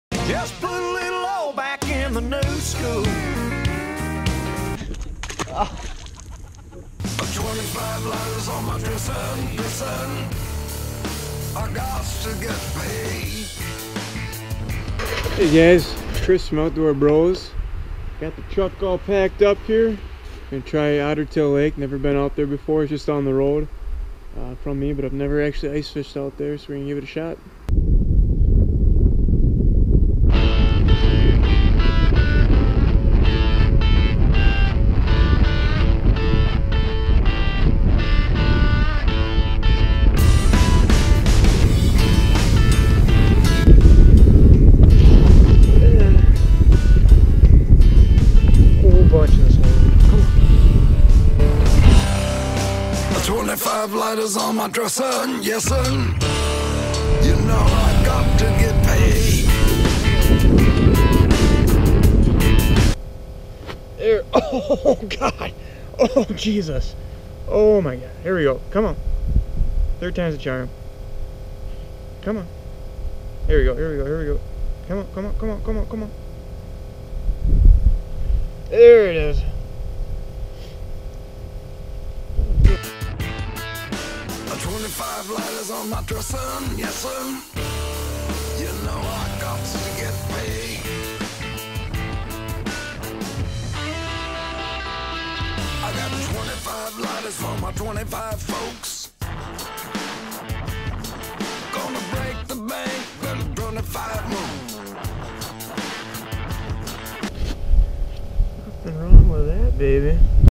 Ottertail Crappies
Smoked them on a #3 Rippin' Rap!